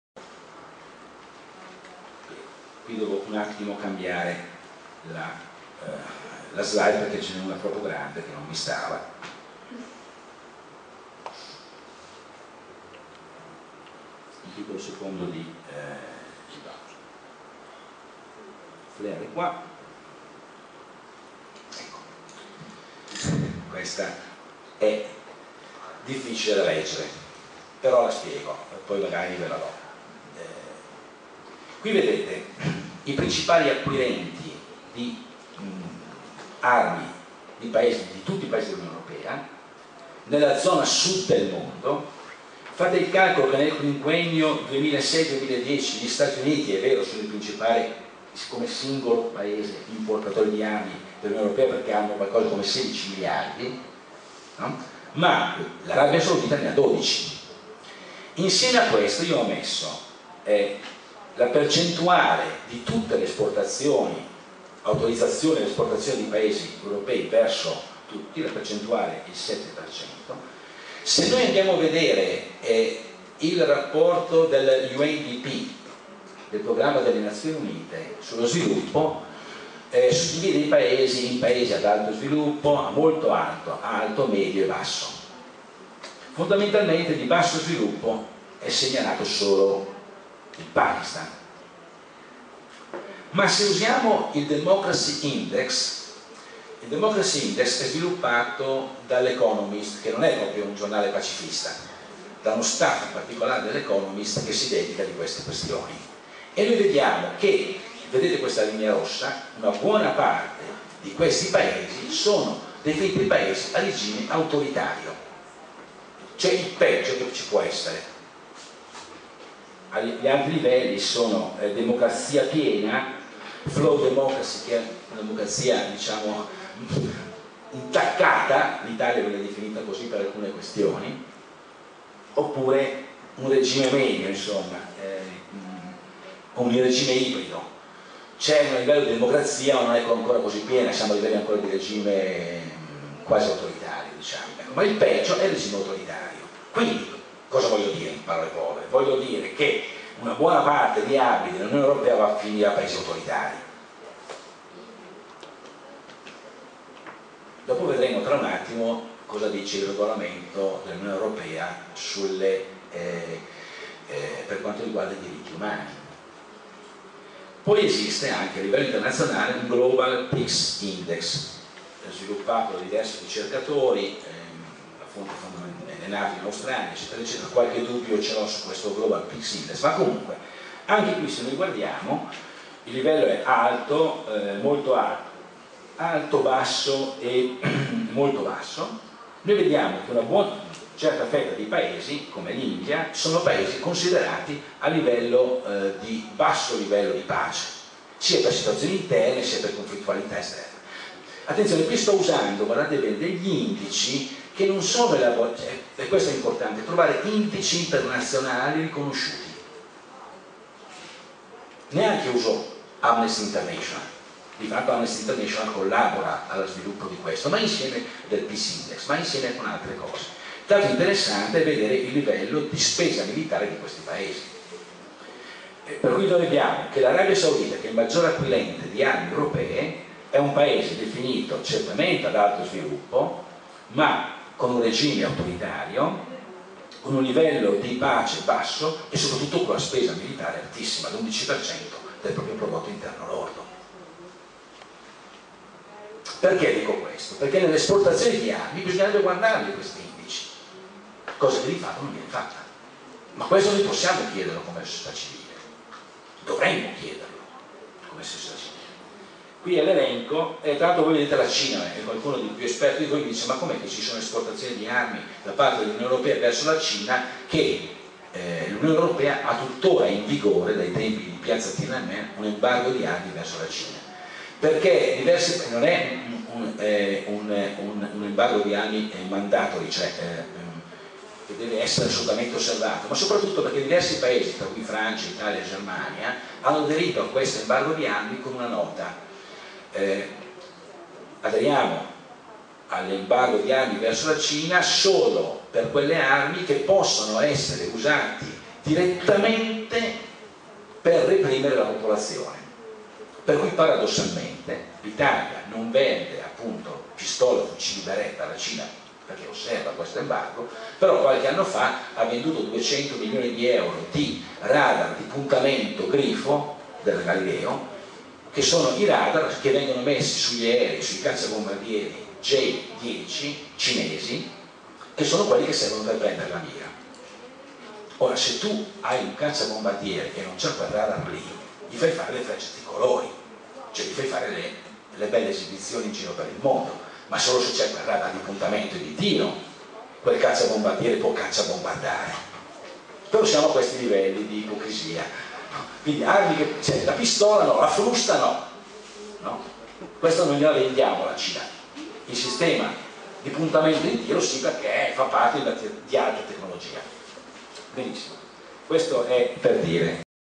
OLE - FLARE 2012 - GIORGIO BERETTA - Parte 7 di 9 - Economy and Arms
Otranto Legality Experience 2012  -  GIORGIO BERETTA - Parte 7 - "Economy and Arms"

SLIDE 23: The first 20 countries of the "Global South" receivers of EU armaments in relation to various Indexes and their Military Expenditures
(For the above Slide and comment see G. Beretta, "Unione Europea, Affari Globali per gli Armamenti" in